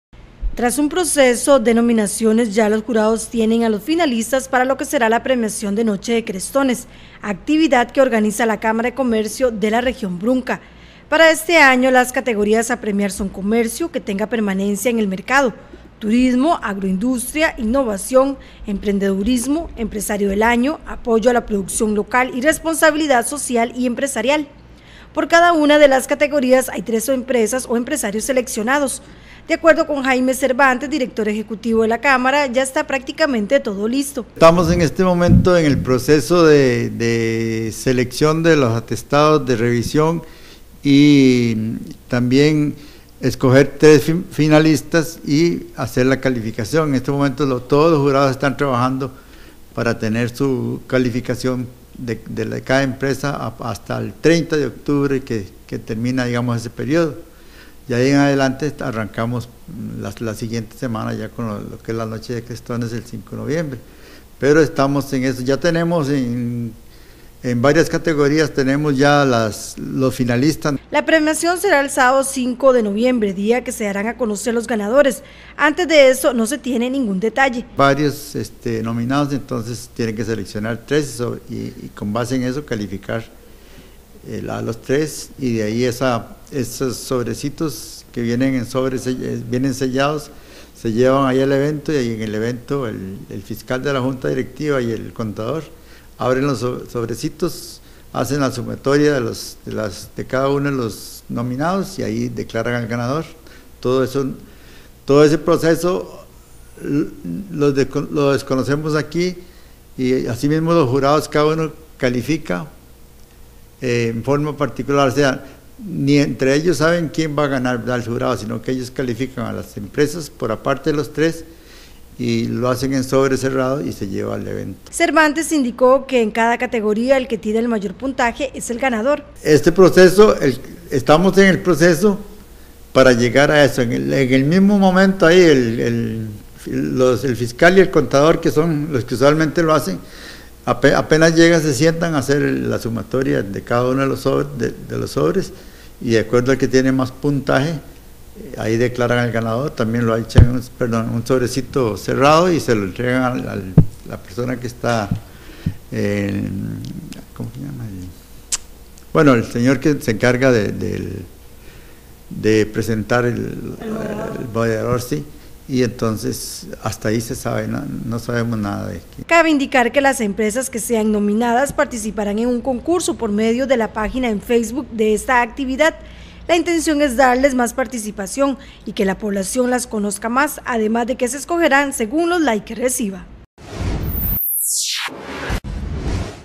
Ya están los nominados para Noche de Crestones
Tras un proceso de nominaciones, ya los jurados tienen a los finalistas para lo que será la premiación de Noche de Crestones, actividad que organiza la Cámara de Comercio de la Región Brunca.